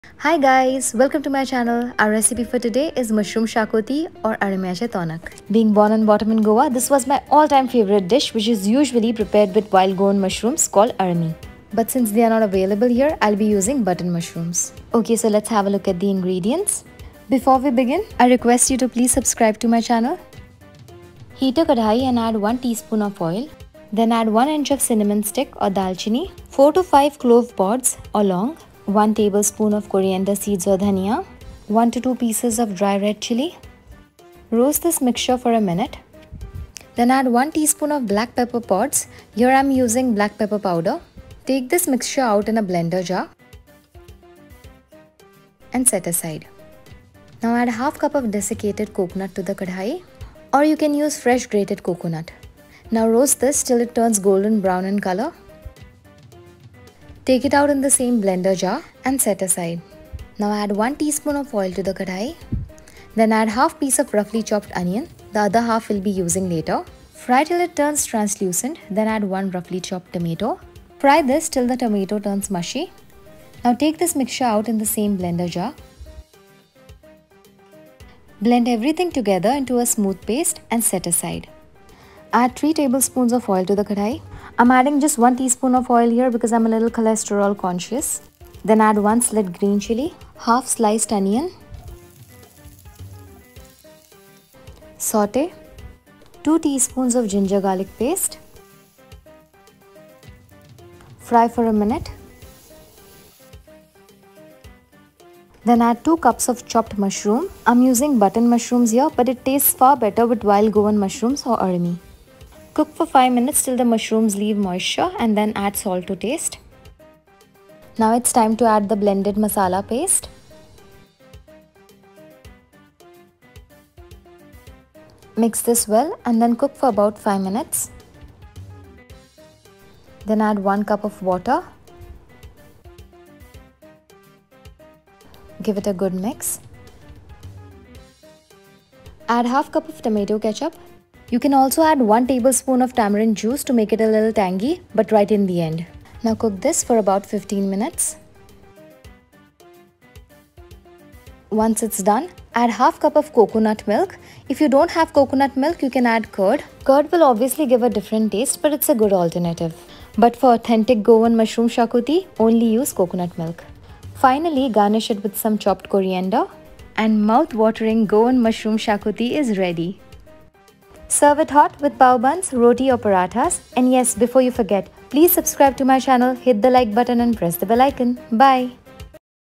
Goan Mushroom Xacuti | Mushroom recipe | mushroom recipes | almiche tonak
BLOOPER ALERT: ADD ONLY 1/2 TBSP OF KETCHUP (NOT 1/2 CUP).. I said “1/2 cup” by mistake 😰🤭

Staying away from home makes you appreciate the food you grew up eating even more. My next dish is decicated to the beautiful state of Goa where I was born and brought up. Our dish for today is Mushroom Xacuti. With the help of hand written recipes from Mom I started cooking and learning the tricks to making Goan dishes. Mushroom Xacuti is my favorite seasonal dish. I’m calling it seasonal because usually to prepare this dish, wild goan mushrooms called Olmi (अळमी or अळमया चा कळया) are used that can be only found in Monsoons. Since they are not available here so I’ll be using Button mushrooms.

Xacuti / Xacutti pronounced as shakuti, is driven from the Portuguese word “Chachuti”. Goan cuisine is igreatly influence by Portuguese colonisation. Sea food, meat and spices are a main ingredients of Goan food.  Xacuti is a Goan curry, made with mushroom or chicken gravy of which is prepared using variety of spices and coconut. Mushroom xacuti is vegetarian version of chicken xacuti. This mouth watering curry can be served with Pav,  plain rice or rotis and naan too…